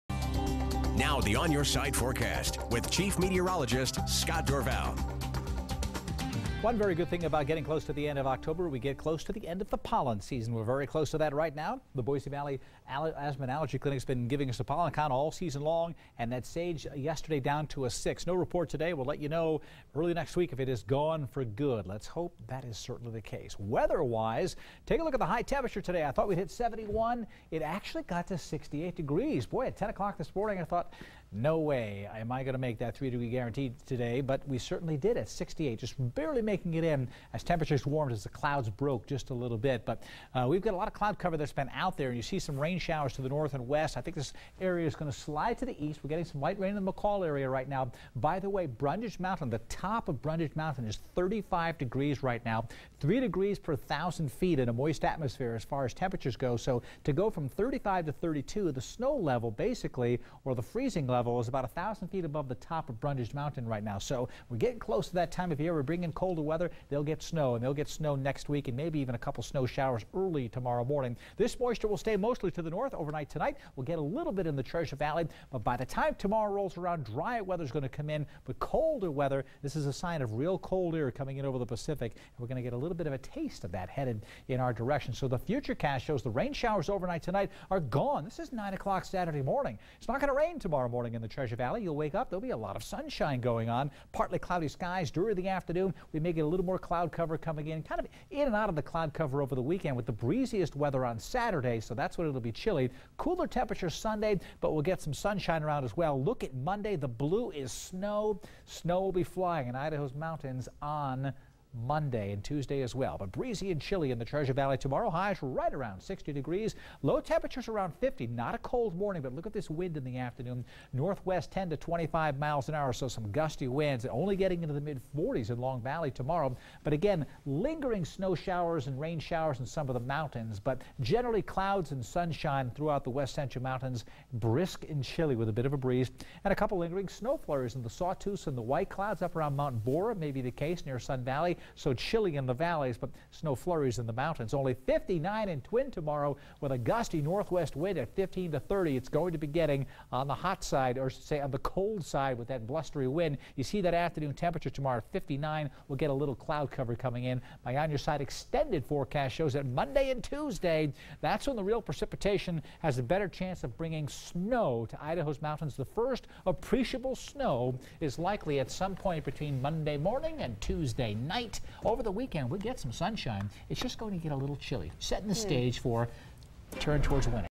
Scott Dorval's On Your Side Forecast - Friday
A blustery weekend ahead... then get ready for mountain snow next week.